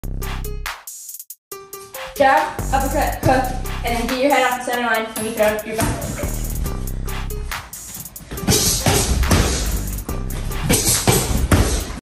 Great Combo for Southpaws!
This combo for southpaws when in an open stance is highly effective.